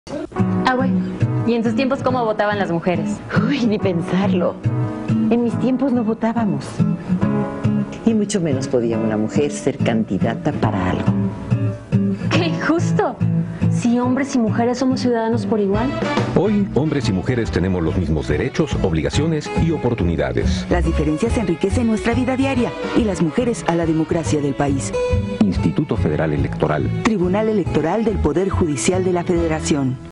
Comerciales mexicanos: IFE-TEPJF 2009
Igualdad entre los ciudadanos en compañía del Tribunal Electoral del Poder Judicial de la Federación